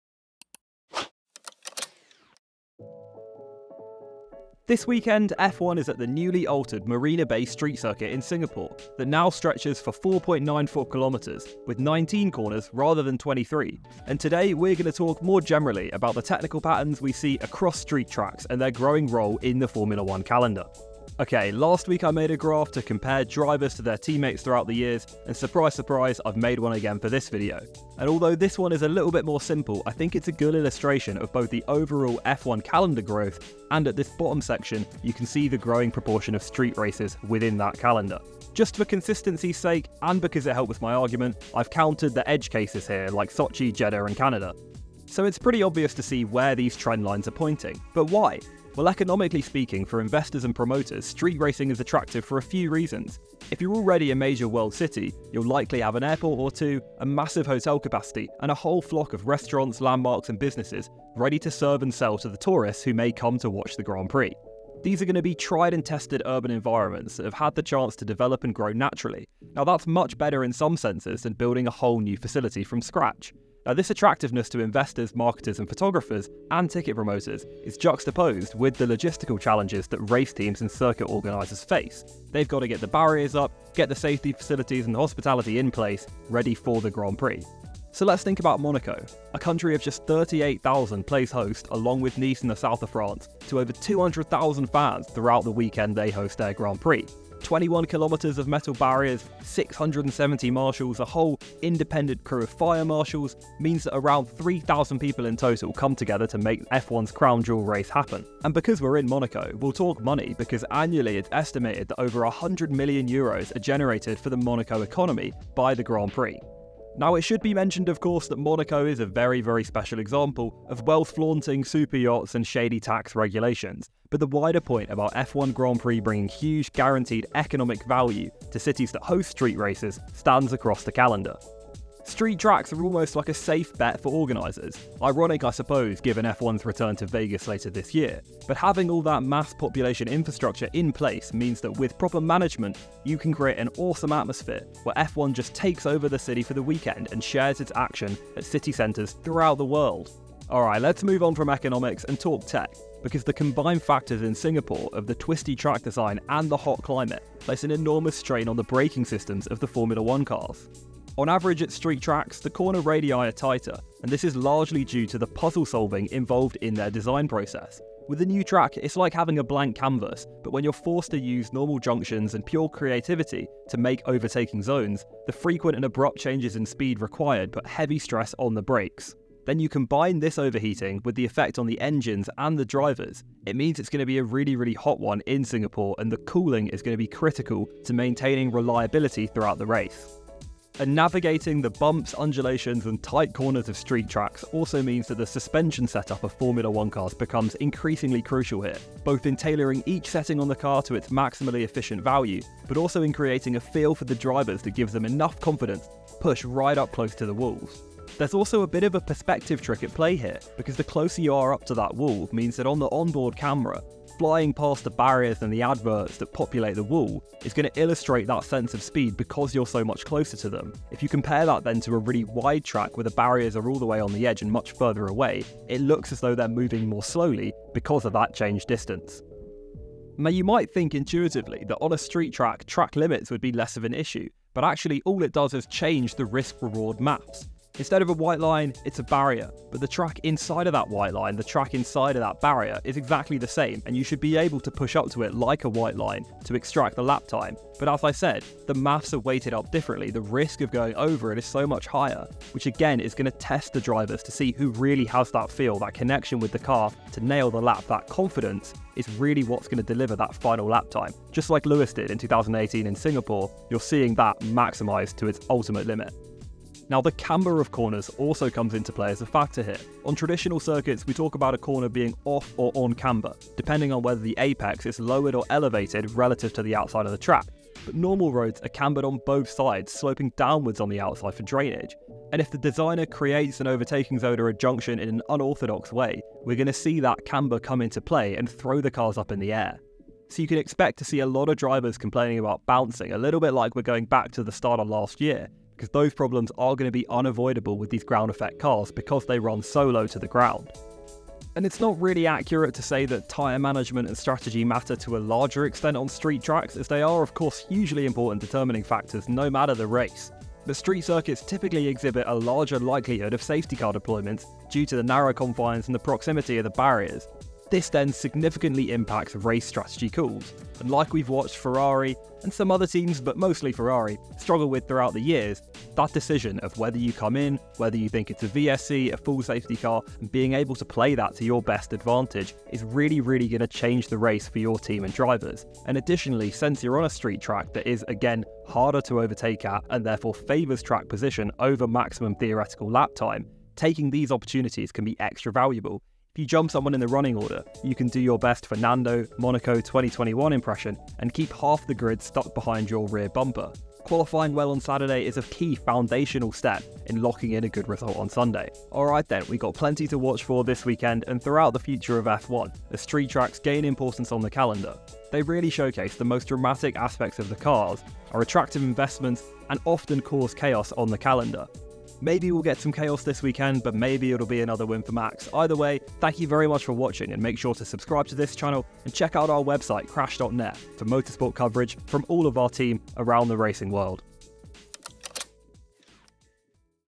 Grand Prix Foundations: Street Tracks in F1
In this video, we explore the economics and physics of street circuits in Formula 1.

We begin by looking at the economics of street circuits. Street circuits are typically much cheaper to build than purpose-built race tracks because they can use existing infrastructure. Additionally, street circuits can generate significant revenue for the host city through tourism and sponsorships.

Next, we will discuss the physics of street circuits. Street circuits are often characterized by tight corners and narrow lanes, which makes it difficult for drivers to overtake. This can lead to more bold strategy choices and chaotic on-track action.